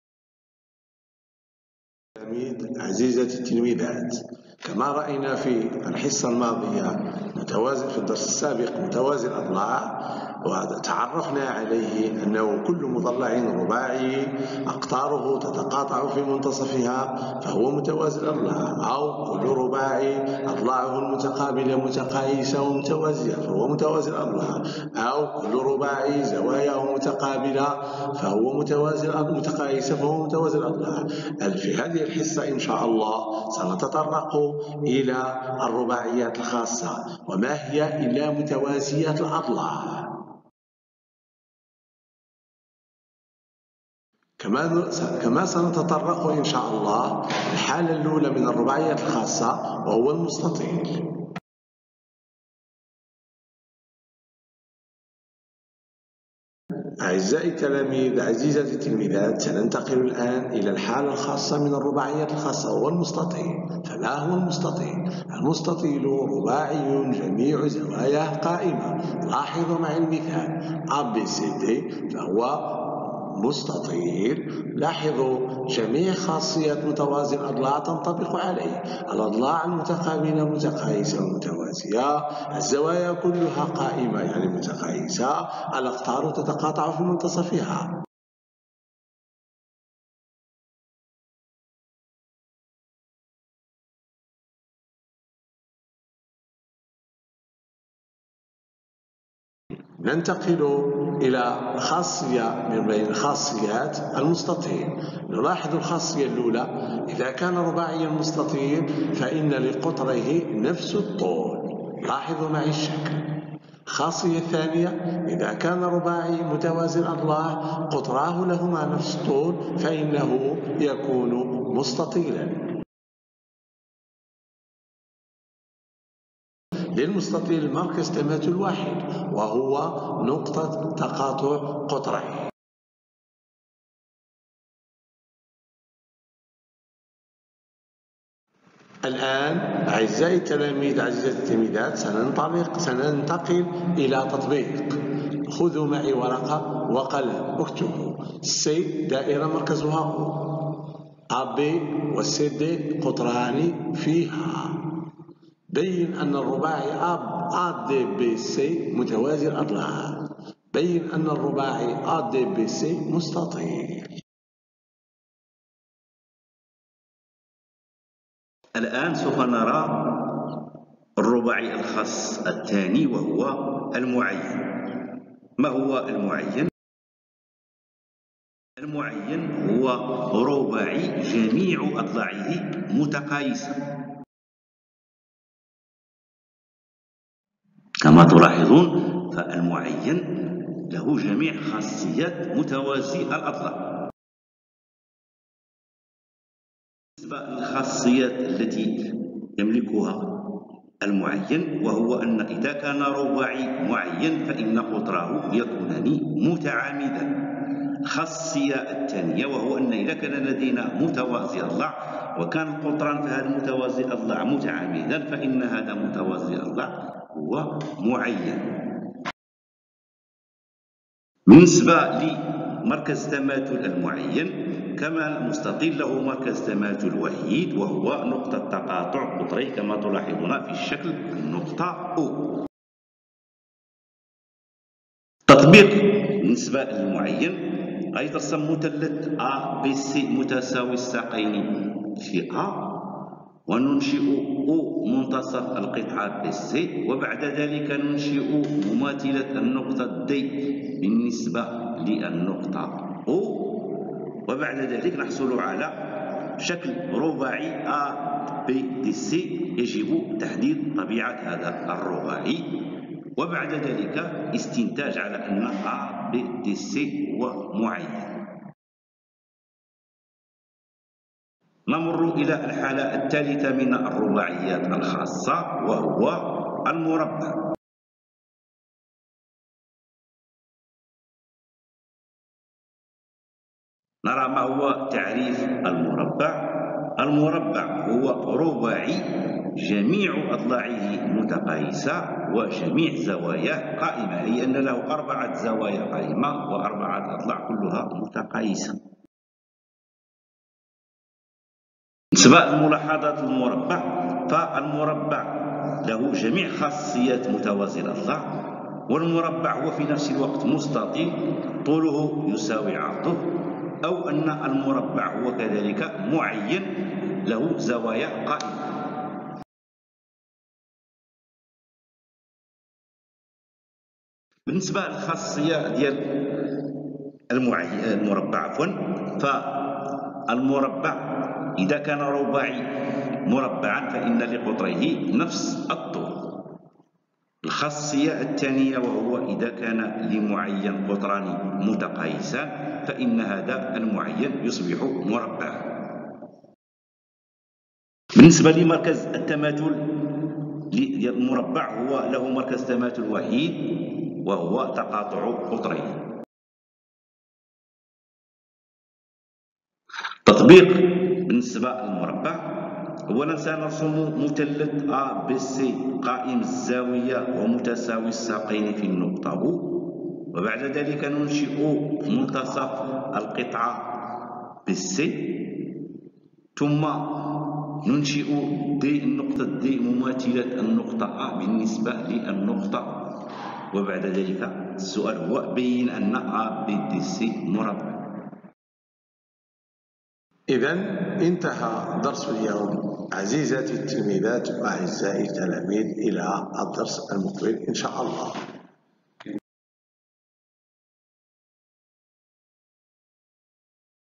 الرباعيات الخاصة
درس لتلاميذ الاولى اعدادي
مادة  الرياضيات
من انجاز الاستاذ : حسن بزيغ
الثانوية الاعدادية التعاون بالعيون